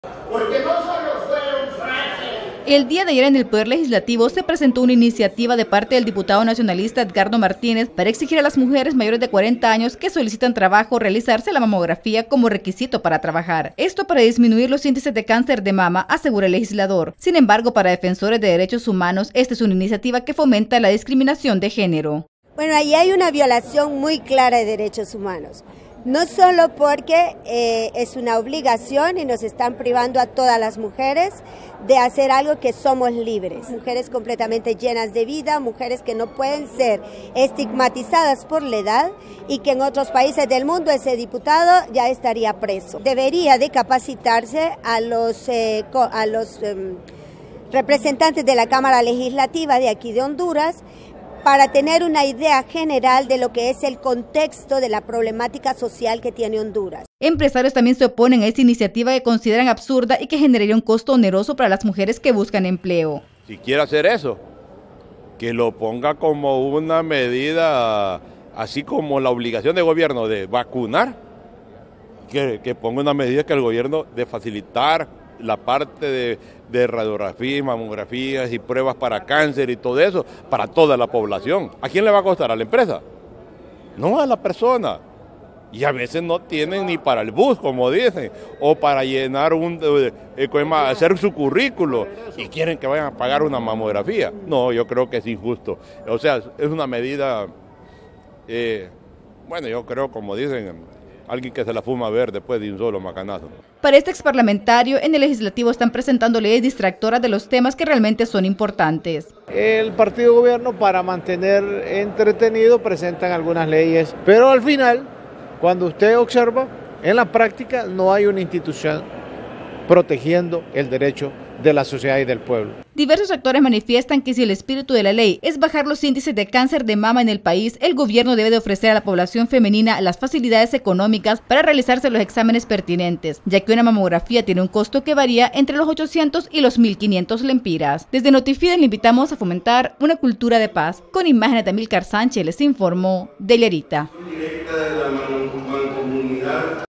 Diversos sectores: leyes presentadas en Congreso son distractores de los temas de interés nacional
Diversos sectores aseguran que leyes presentadas en el Congreso, como la de exigir una mamografía a las mujeres para solicitar empleo, son solamente distractores de los temas de interés nacional